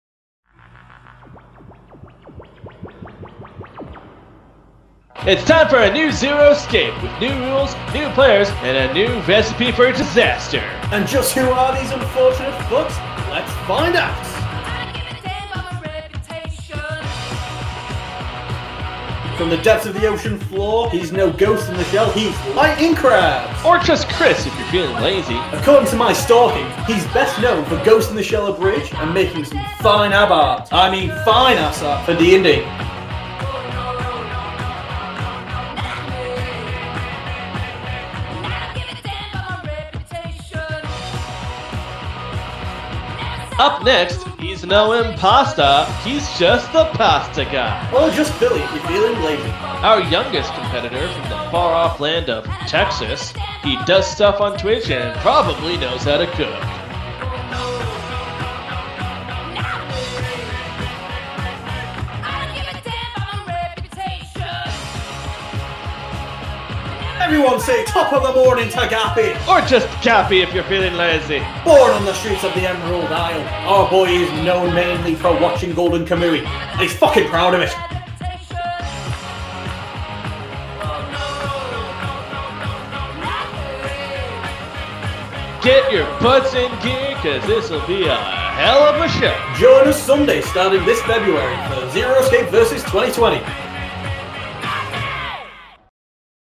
Zero Escape VS 2020 ➤ Player Introductions
This video introductions the third season's players.